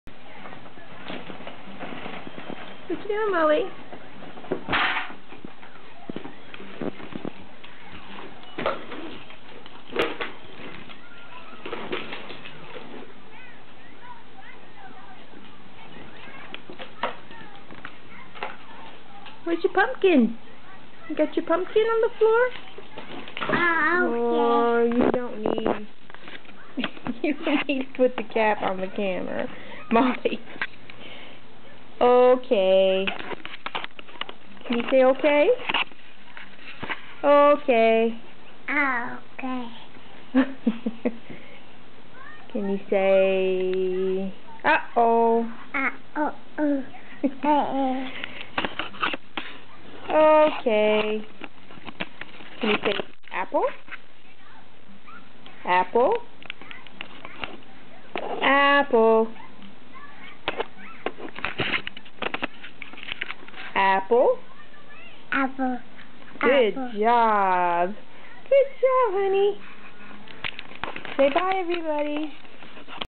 Molly's showing us her vocabulary skills :)
Molly has learned some words, and she is doing quite well with them. Just wanted to share.